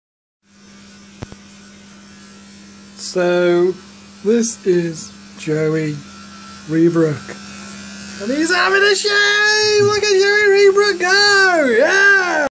My flatmate being a TIT and catching me shaving
My flatmate being a dick. And catching me shaving. There is a story behind joey reabrook lmao. Just think only way is essex but in shrewsbury lmao.